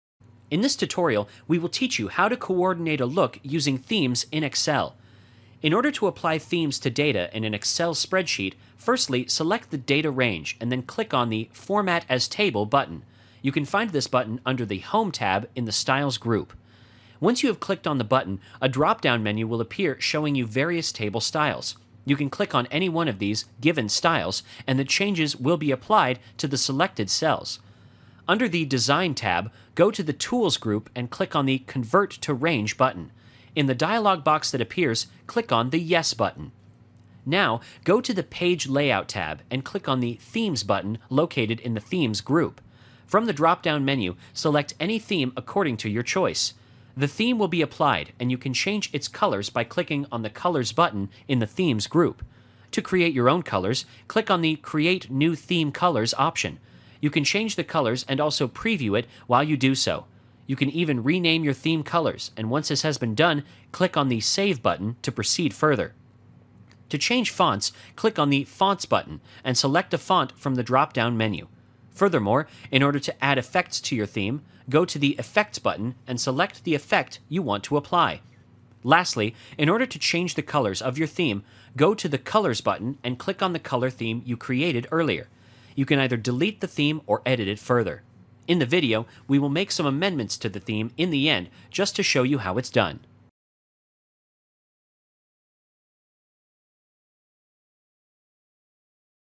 How to coordinate a look using themes in Excel 2010
Have problems coordinating a look while using themes in Excel? Follow this tutorial and learn how to do so in a few simple steps.

Microsoft Excel is an extremely helpful program when it comes to arranging data in the form of spreadsheets or carrying out mathematical calculations. One can also use themes in excel in order to make it look more attractive; add some color and eye catching features to it. Customizing the sheet gives it a personalized approach.

In this Excel tutorial, we will teach you how to coordinate a look using themes in Excel.

Step 1: Using the "format as table icon" option
One might want to give their data a more consistent look while working in an excel sheet and thus can apply a theme to it. We will teach you how to make and modify a theme in this Excel tutorial. In order to do so, first select the data range and then click on the "format as table icon". This will be given under the "home" tab in the "styles" group.

Step 2: Using the "convert to range" button
Once you have clicked on this option, a drop down menu will appear displaying a variety of tables and table styles. Click on any one of these as per your requirement and preference and the data will be organized accordingly.
Next, under the "design" tab, go to the "tools" group and click on the "Convert to range" button. This will cause a dialogue box to appear where you must click on the "yes" button.

Step 3: Creating new theme colors
Once the above steps have been completed, go to the Page layout" tab and click on the "Themes" button which is given in the "themes" group. From the drop down menu, you will be able to see the list from which you can select and apply any one of the various themes in excel.
After the theme has been applied, change the colors by clicking on the "colors" button. In order to create your own colors, click on the "Create new theme colors" option.

Step 4: Save the new theme
You can change the colors and you can also see the preview while you change them. At the bottom, you can also give a name to your theme colors. After you have done that, click on the "save" button to exit.

Step 5: Apply Fonts and Effects
Fonts can also be changed by clicking on the "fonts" button. Select the font from the drop down menu. Similarly, you can also give effects to your theme. Go to the "effects" button and select the effect you want to apply.

Step 6: Edit the colors of the theme
If you want to change the colors of your theme, go to the "colors" button and click on the name of the color theme that you had created earlier.